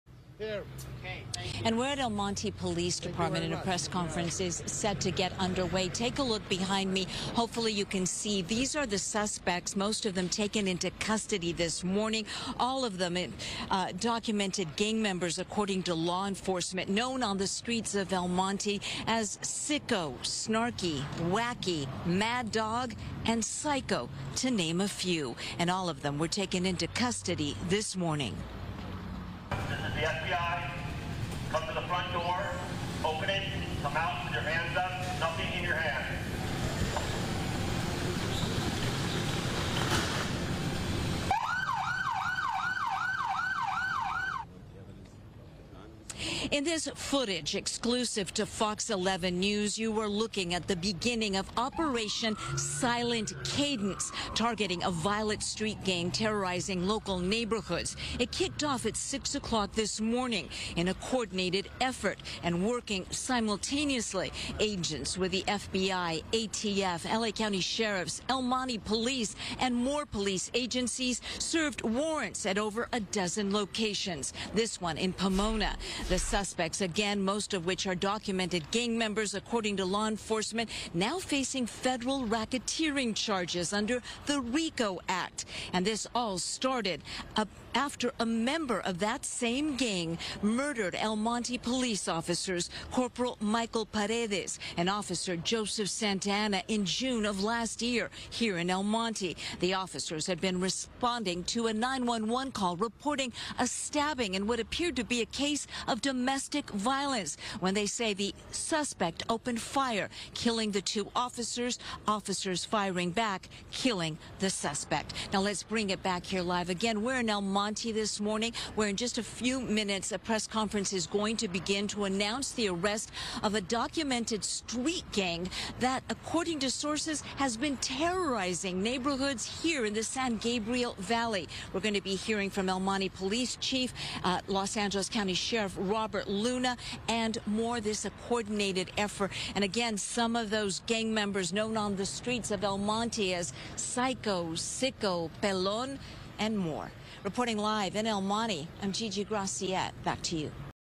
Nearly a dozen documented gang members arrested in targeted FBI raids in El Monte
Several gang members and associates in the San Gabriel Valley were arrested across El Monte Wednesday as part of targeted raids, officials said.